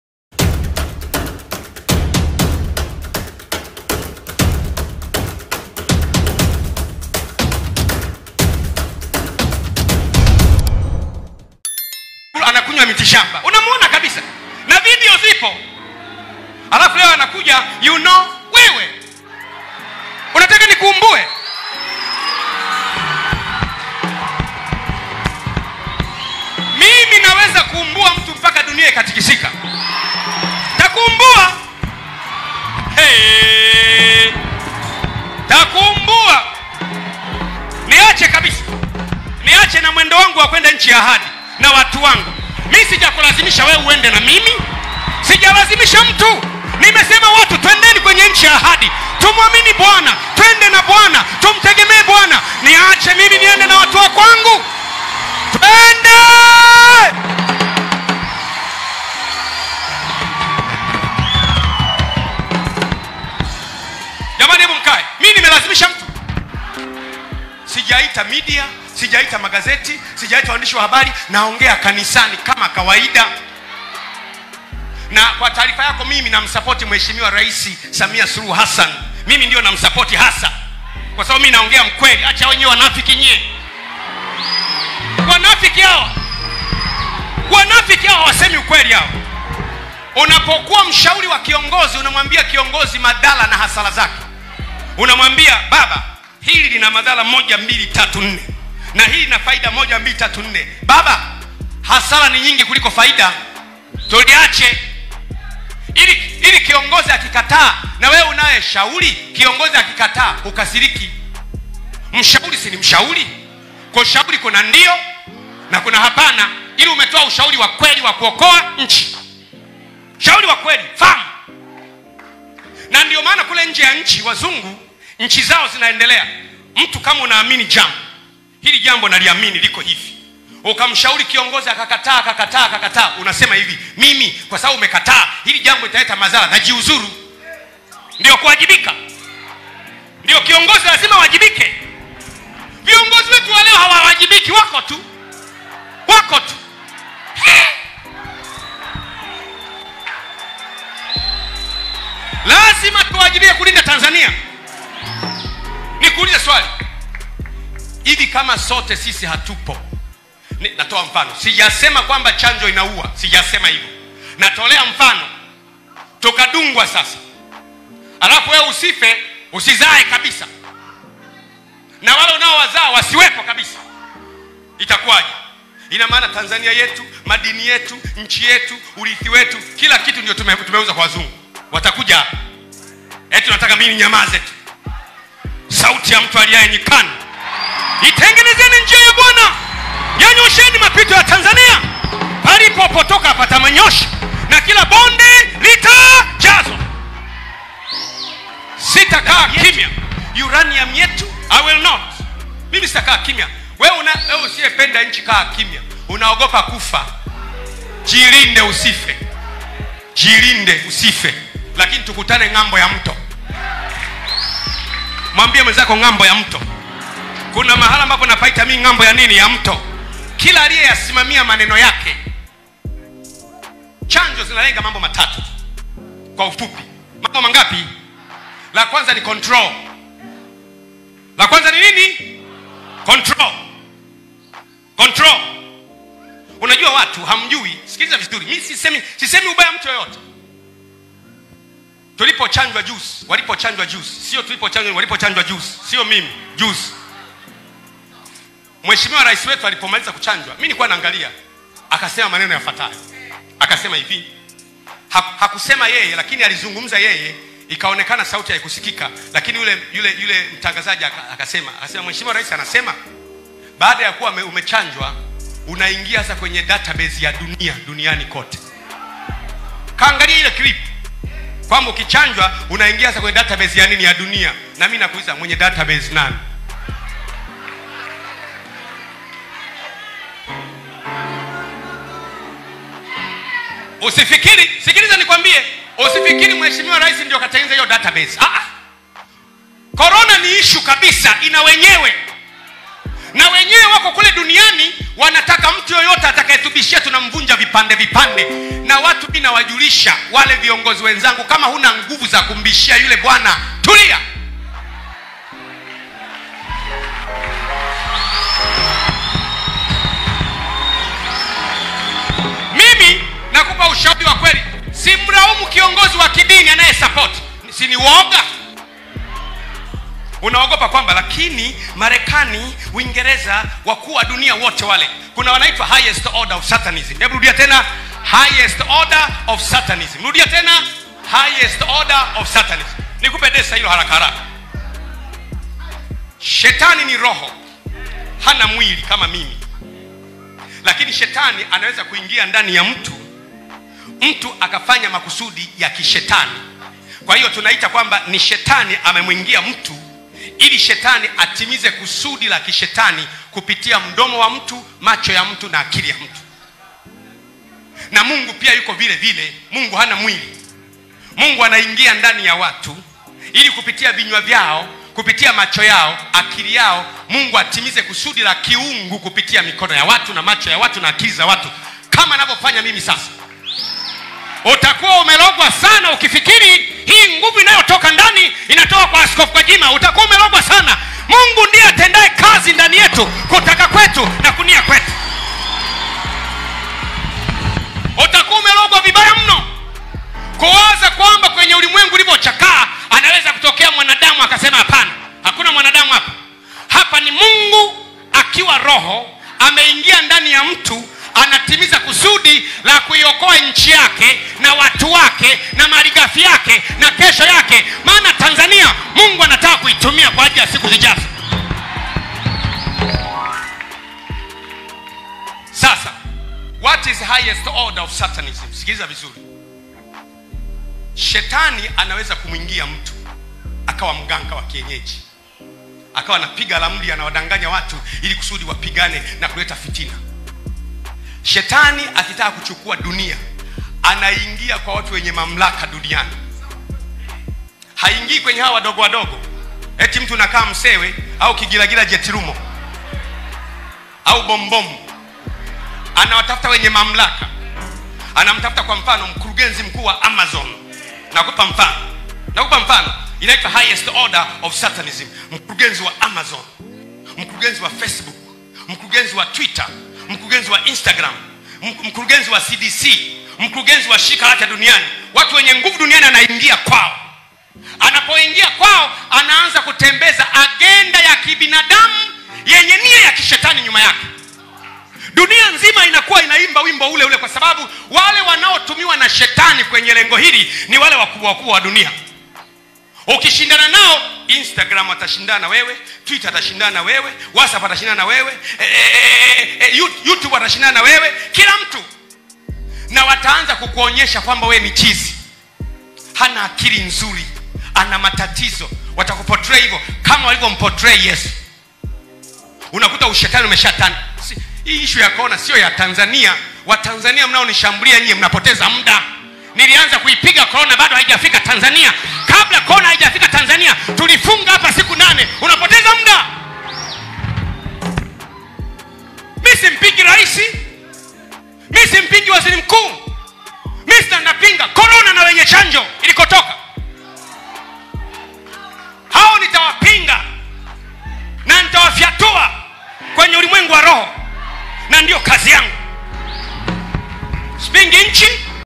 DUH.! GWAJIMA AFICHUA SIRI HII NZITO TENA
DUH.! GWAJIMA AFICHUA SIRI HII NZITO TENA.!,AMUONYA VIKALI RAIS SAMIA UBAYA WAKE BILA KUFICHA TAZAMA